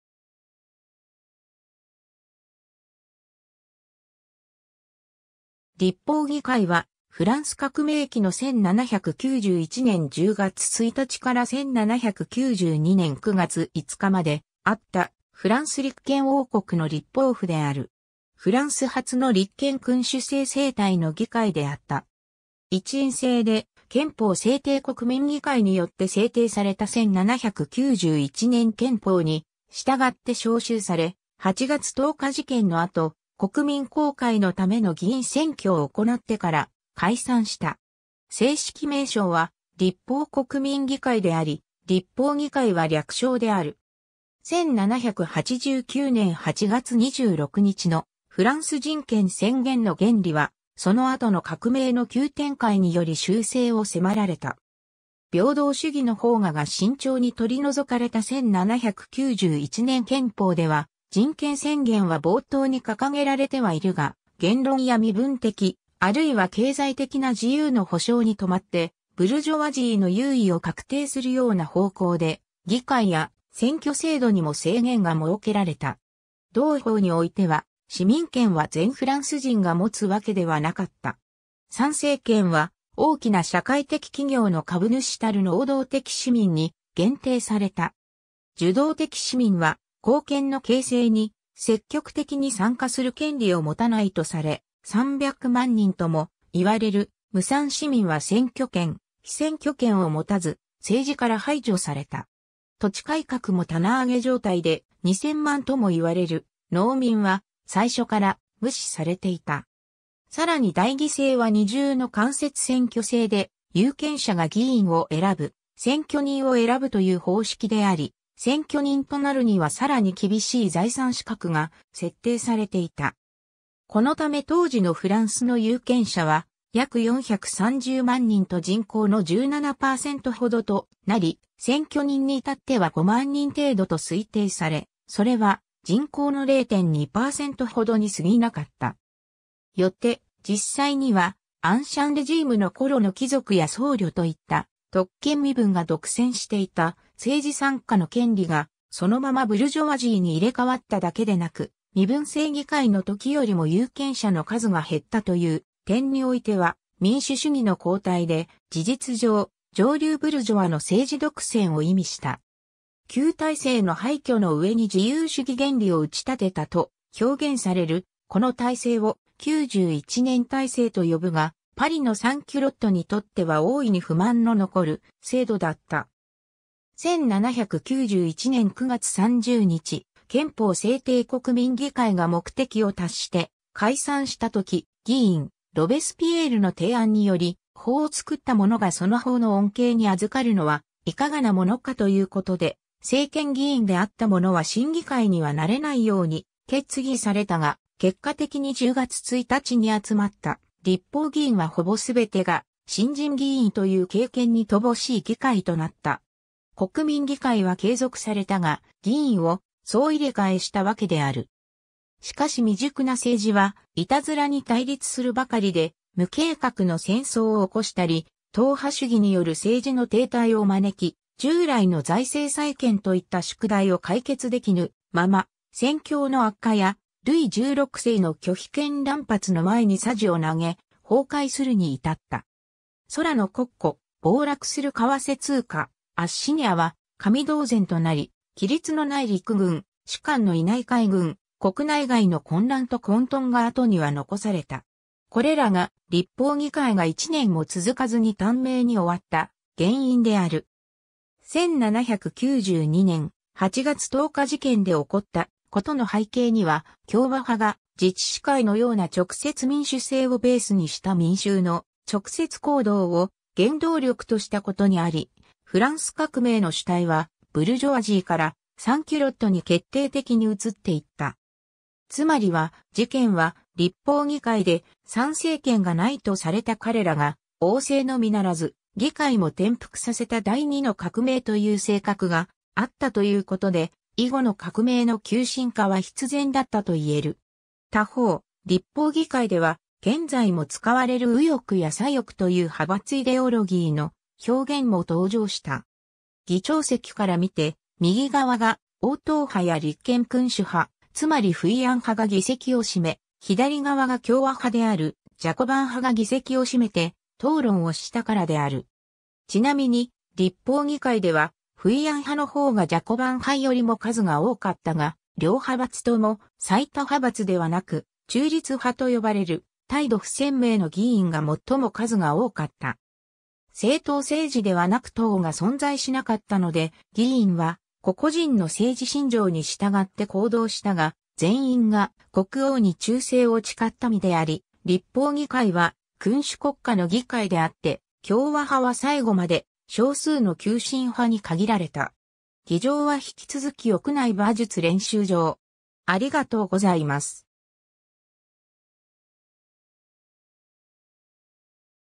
立法議会
立法議会（りっぽうぎかい、仏: Assemblée législative）は、フランス革命期の1791年10月1日から1792年9月5日まであった、フランス立憲王国の立法府である。
フランス初の立憲君主制政体の議会であった。
一院制で、憲法制定国民議会によって制定された1791年憲法に従って召集され、8月10日事件の後、国民公会のための議員選挙を行ってから解散した。
正式名称は立法国民議会（仏: Assemblée nationale législative）であり、立法議会は略称である。
 1789年8月26日のフランス人権宣言の原理は、その後の革命の急展開により修正を迫られた。
平等主義の萌芽が慎重に取り除かれた1791年憲法では、人権宣言は冒頭に掲げられてはいるが、言論や身分的、あるいは経済的な自由の保証に留まって、ブルジョワジーの優位を確定するような方向で、議会や選挙制度にも制限が設けられた。
同法においては、市民権は全フランス人が持つわけではなかった。
参政権は「大きな社会的企業の株主たる」能動的市民（男性）に限定された。
受動的市民は「公権の形成に積極的に参加する」権利を持たないとされ、300万人とも言われる無産市民は選挙権・被選挙権を持たず、政治から排除された。
土地改革も棚上げ状態で、2000万ともいわれる農民は最初から無視されていた。
さらに代議制は二重の間接選挙制で、有権者が議員を選ぶ選挙人を選ぶという方式であり、選挙人となるにはさらに厳しい財産資格が設定されていた。
このため当時のフランスの有権者は約430万人と人口の17%ほどとなり、選挙人に至っては5万人程度と推定され、それは人口の0.2%ほどに過ぎなかった。
よって実際には、アンシャン・レジームの頃の貴族や僧侶といった特権身分が独占していた政治参加の権利が、そのままブルジョワジーに入れ替わっただけでなく、身分制議会のときよりも有権者の数が減ったという点においては民主主義の後退で、事実上、上流ブルジョワの政治独占を意味した。
「旧体制の廃墟の上に自由主義原理を打ち立てた」と表現されるこの体制を、91年体制と呼ぶが、パリのサン・キュロットにとっては大いに不満の残る制度だった。
 1791年9月30日、憲法制定国民議会が目的を達して解散した時、議員ロベスピエールの提案により、法を作った者がその法の恩恵に与るのはいかがなものかということで、制憲議員であった者は新議会にはなれないように決議されたが、結果的に10月1日に集まった立法議員はほぼすべてが新人議員という経験に乏しい議会となった。
「国民議会」は継続されたが、議員を総入れ替えしたわけである。
しかし未熟な政治は、いたずらに対立するばかりで、無計画の戦争を起こしたり、党派主義による政治の停滞を招き、従来の財政再建といった宿題を解決できぬまま、戦況の悪化や、ルイ16世の拒否権乱発の前にさじを投げ、崩壊するに至った。
空の国庫、暴落する為替通貨、アッシニアは紙同然となり、規律のない陸軍、士官のいない海軍、国内外の混乱と混沌が後には残された。
これらが立法議会が1年も続かずに短命に終わった原因である。
 1792年、8月10日事件で起こったことの背景には、共和派が自治市会（パリ・コミューン）のような直接民主制をベースにした民衆の直接行動を原動力としたことにあり、フランス革命の主体は、ブルジョワジーからサン・キュロット（別の言い方をすればプロレタリアート）に決定的に移っていった。
つまりは事件は、立法議会で参政権がないとされた彼らが、王政のみならず議会も転覆させた第二の革命という性格があったということで、以後の革命の急進化は必然だったと言える。
 他方、立法議会では、現在も使われる右翼や左翼という派閥イデオロギーの表現も登場した。
議長席からみて右側が王党派や立憲君主派、つまりフイヤン派が議席を占め、左側が共和派であるジャコバン派（ジロンド派を含む）が議席を占めて、討論をしたからである。
ちなみに立法議会では、フイヤン派の方がジャコバン派よりも数が多かったが、両派閥とも最多派閥ではなく、中立派と呼ばれる態度不鮮明の議員が最も数が多かった。
政党政治ではなく党が存在しなかったので、議員は個々人の政治信条に従って行動したが、全員が国王に忠誠を誓った身であり、立法議会は君主国家の議会であって、共和派は最後まで少数の急進派に限られた。
 議場は引き続き屋内馬術...